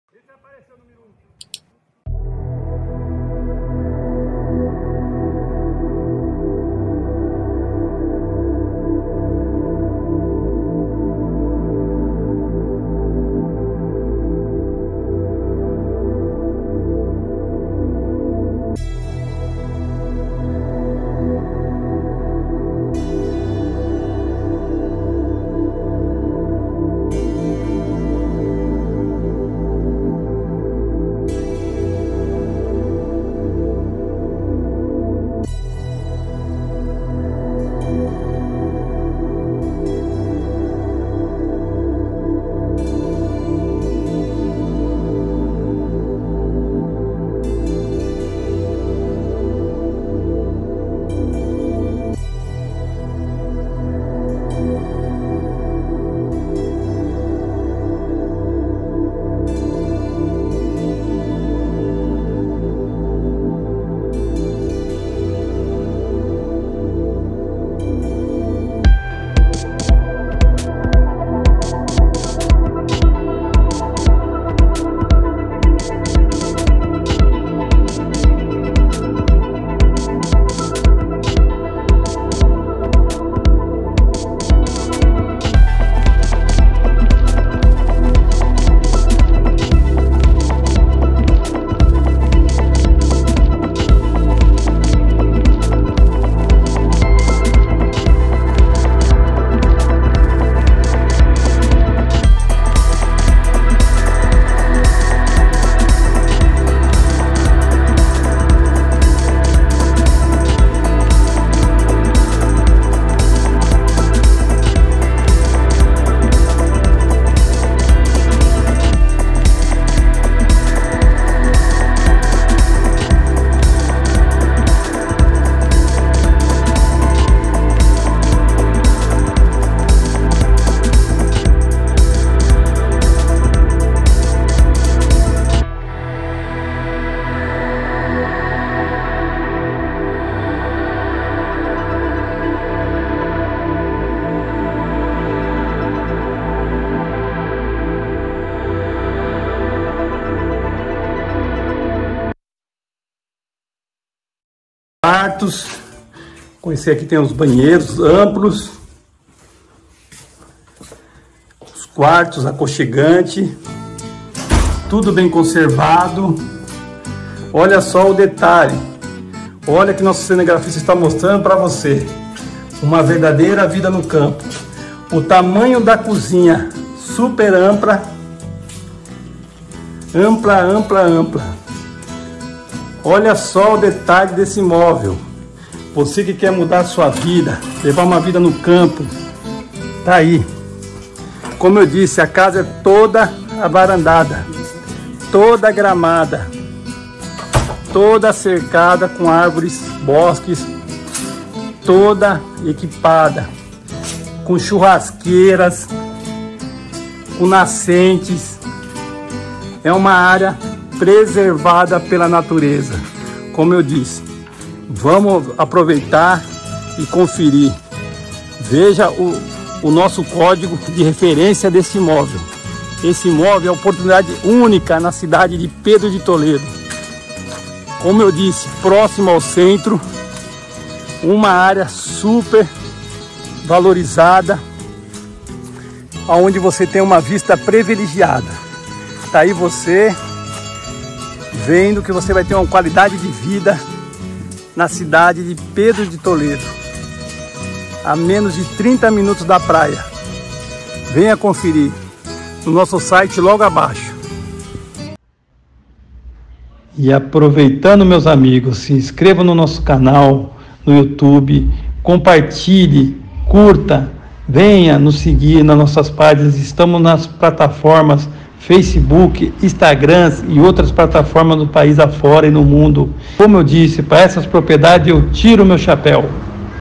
REF. 568, INVESTIMENTO PERFEITO EM PEDRO DE TOLEDO - SP POR R$420.000,00
REF. 568, CONTRATO DE COMPRA E VENDA + ESCRITURA ANTIGA, PEDRO DE TOLEDO - SP.

UMA BELA E AMPLA CHÁCARA EM PEDRO DE TOLEDO - SP, COM 14.958,56 M², HÁ 1.500 M² DA CIDADE, VISTA PANORAMICA DE TODA A CIDADE, COM LAGO, NASCENTES, ARVORES NATIVAS, FRUTEIRAS EM GERAL, EM SISTEMA AGROFLORESTA.

UM INVESTIMENTO DE QUALIDADE DE VIDA E DE RENDIMENTOS FINANCEIROS.

VENHA INSVESTIR! INVISTA NA SUA QUALIDADE DE VIDA! INVISTA NA SUA FAMILIA!